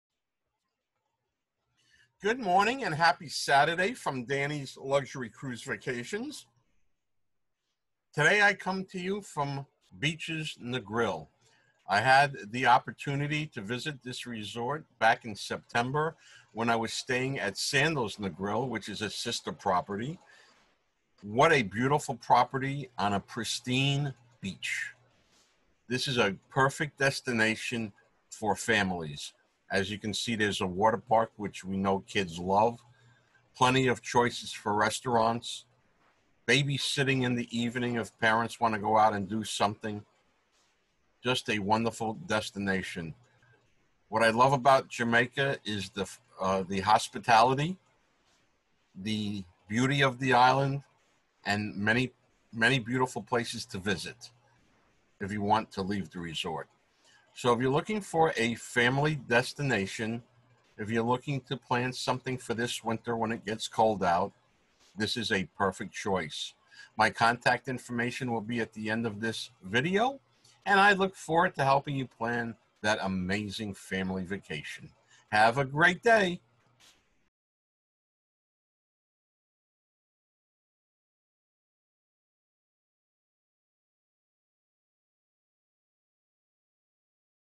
Beaches Negril
Beaches Negril is situated on a stretch of 7 mile Beach. It is a perfect destination for a family vacation. It is all inclusive with a water park on property.